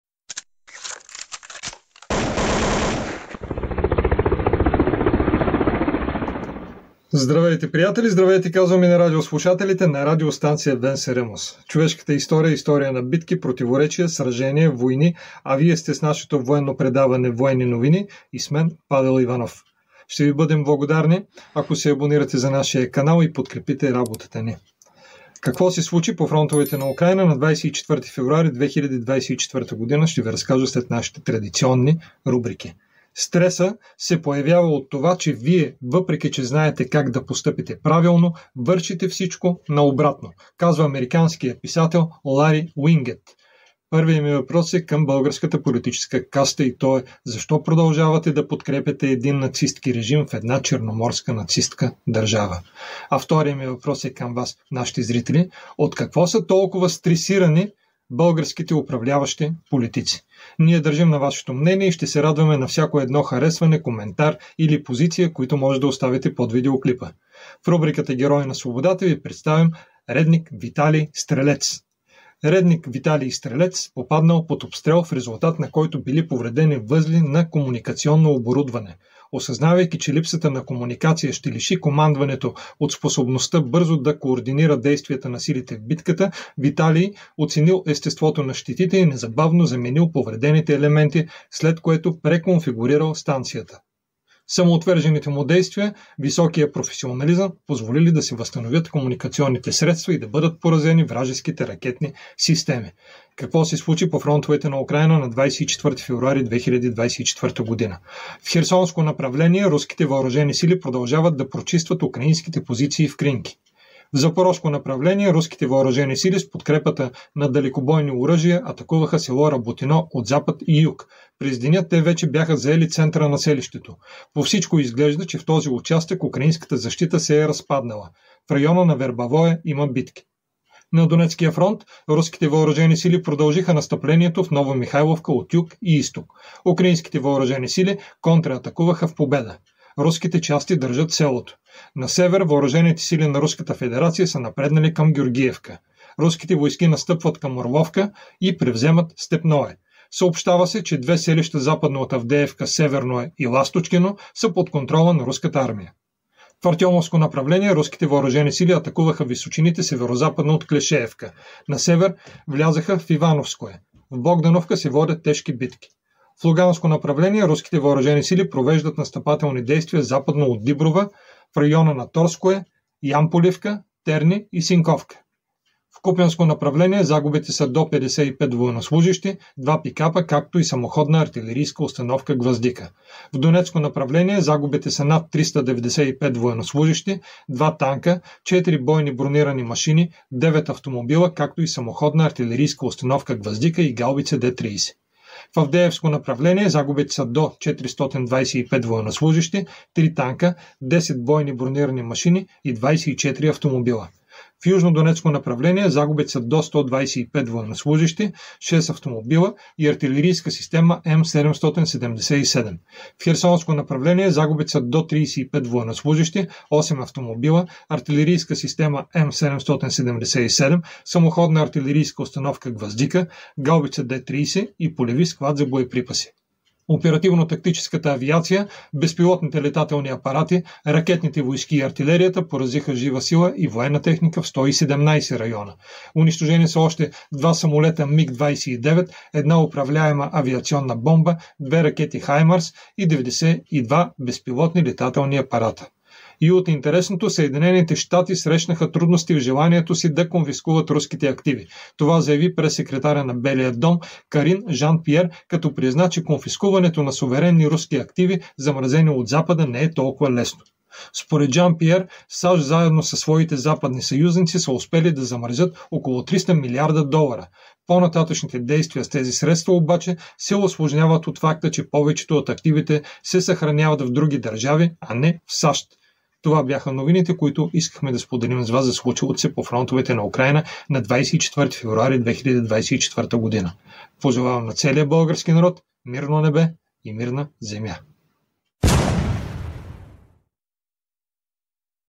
Военни новини с Павел Иванов: Какво се случи на 24 февруари 2024 г. по фронтовете на Украйна
Военни новини от изминалия ден с Павел Иванов: Какво се случи на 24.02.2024 г. по фронтовете на Украйна - информацията, която останалите медии ви спестяват.